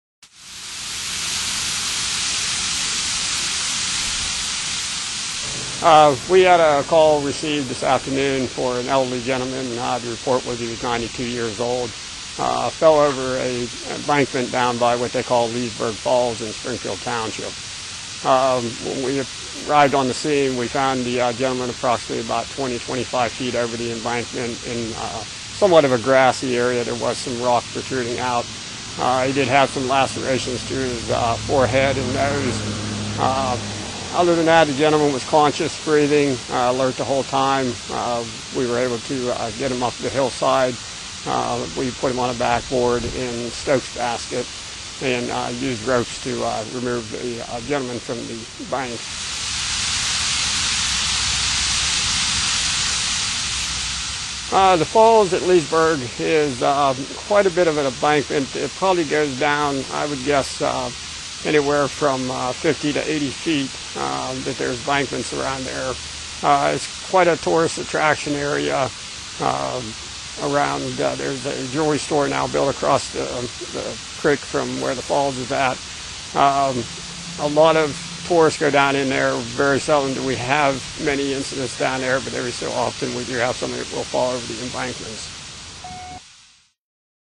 Elderly Man Rescued At Leesburg Falls [4/3/2010]
Mercer County – 04/03/10About 3:40 this afternoon, Springfield Township VFD was dispatched to Leesburg Falls on Falls Road for an elderly man (reported to be 92 years-old ) who had fallen over the hill near the falls. Fire Chief Stephen Rae says the man had fallen approx. 20-25 feet next to the falls and was suffering with cuts to the head and bleeding. The man was conscious and was able to talk with rescuers who used a stokes basket to bring the man up the embankment and to a waiting ambulance. The falls is a local tourist attraction and according to Rae, it is rare for someone to fall there but they have had to respond to a few calls there over the years.